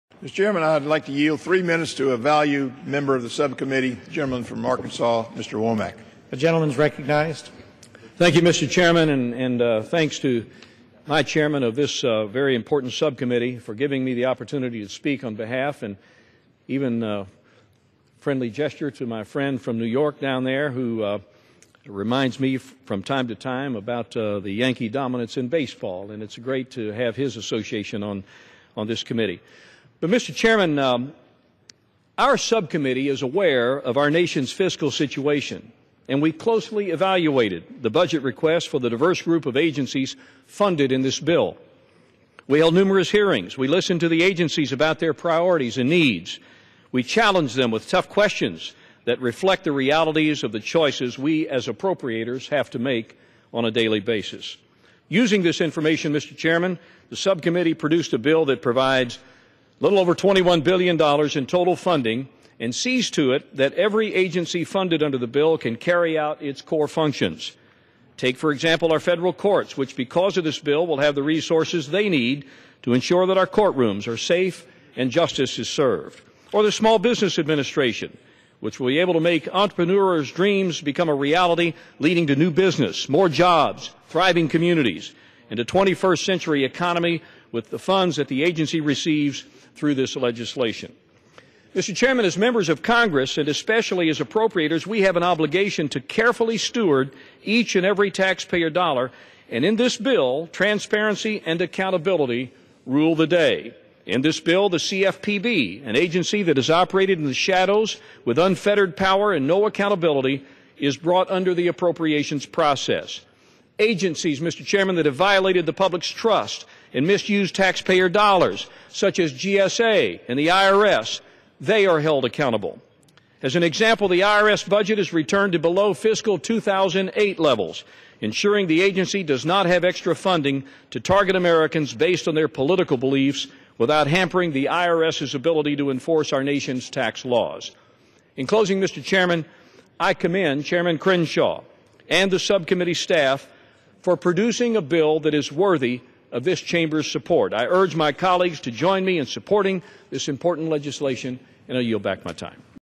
ICYMI: Womack Addresses FSGG Appropriations on House Floor
On Monday, July 14, 2014, in a speech given on the House Floor, Congressman Steve Womack (AR-3) spoke in support of H.R. 5016, the Financial Services and General Government Appropriations Act, 2015.  H.R. 5016 was subsequently passed  by the House on July 16, 2014.  The bill provides funding for the Treasury Department, the Judiciary, the Small Business Administration, the Securities and Exchange Commission, and several other agencies for fiscal year 2015.